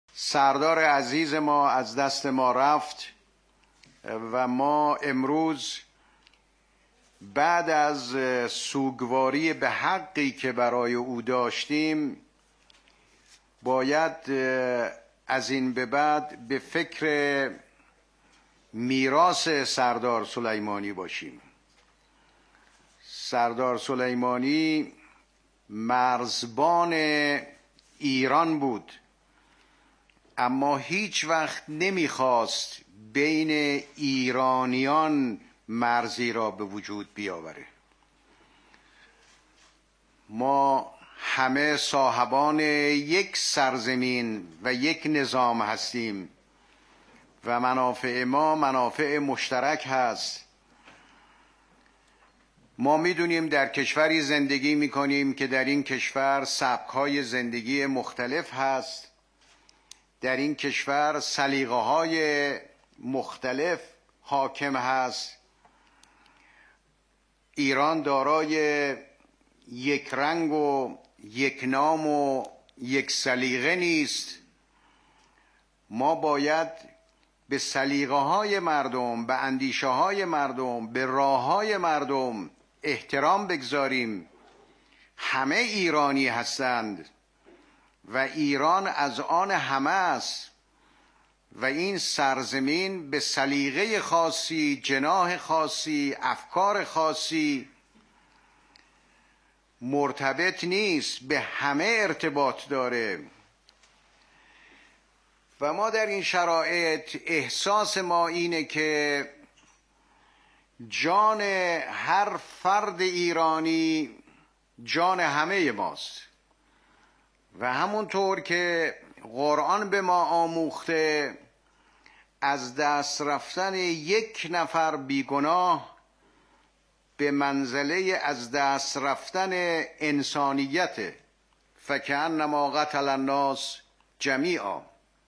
روحانی: باید میراث سردار سلیمانی را حفظ کنیم
رییس جمهور در جلسه هیات دولت:
باید میراث سردار سلیمانی را حفظ کنیم؛ او مرزبان ایران بود اما هیچ‌وقت نمی‌خواست بین ایرانیان مرزی به وجود بیاورد
همه ایرانی هستیم و ایران از آن همه است؛ باید به سلیقه‌ها ، اندیشه‌ها و راه‌های مردم احترام گذاشت